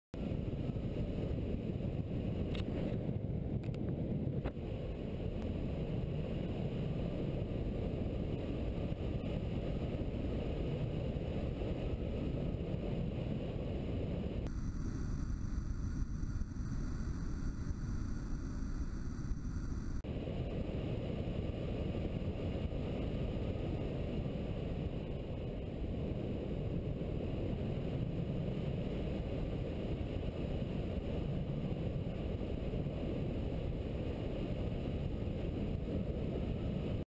Bastrop Maze - Hangglider's View
This was a short flight on a challenging day from Smithville to Utley Airfield.  Great fly day friday with Flytexas